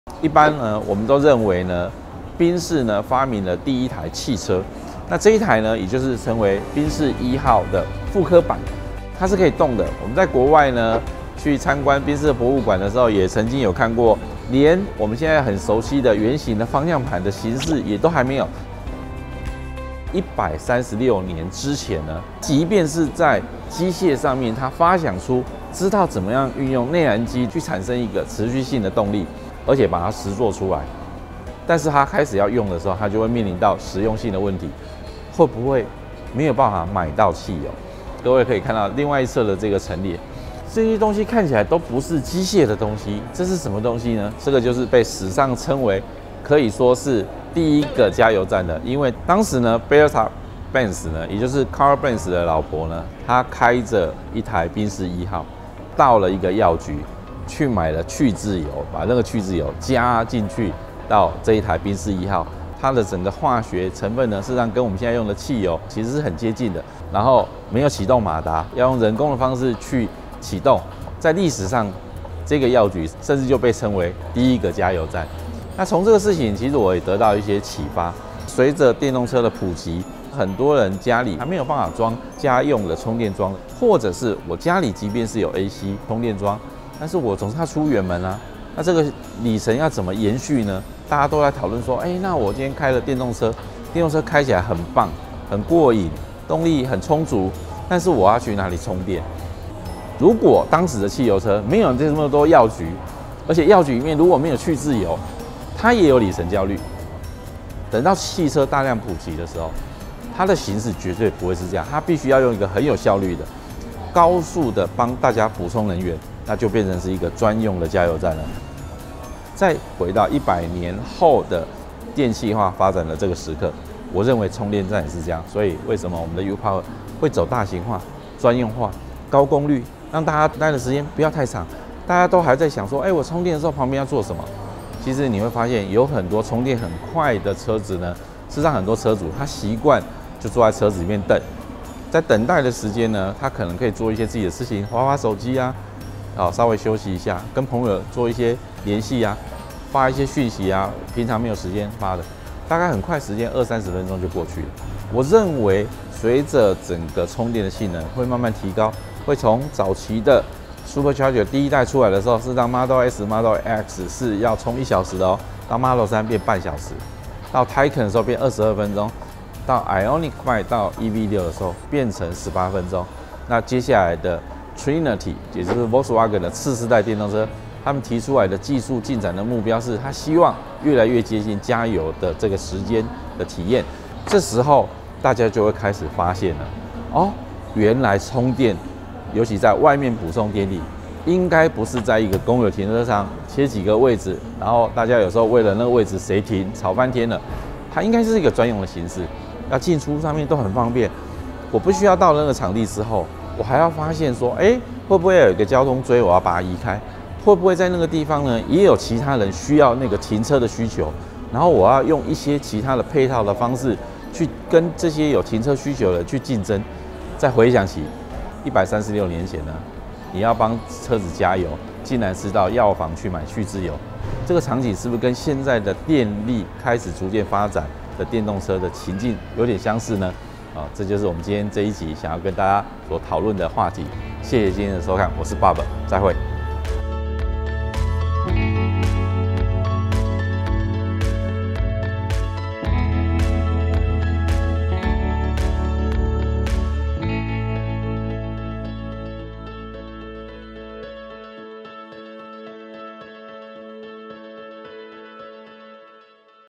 【Bob聊電】高效率的充電站「不該是停車位」：從汽車的誕生，反思電動車充電生態、未來快充站願景 (中文字幕)｜Bertha Benz Patent-Motorwagen｜佔用車格｜U-CAR 專題企劃
進入電動車時代，Bob藉由台灣賓士展出史上首款汽車「Benz Patent 1」複製車的機會，分享消費需求由「加油」轉換為「充電」的過程與願景。
【Bob聊電】
※影片中之無人機禁(限)航區空拍作業經申請限制排除，由交通部民航局、各縣市空域權責單位、臺北近場管制塔臺與高雄近場管制塔臺 核准後執行。
講解：Bob
攝影/剪輯：Yan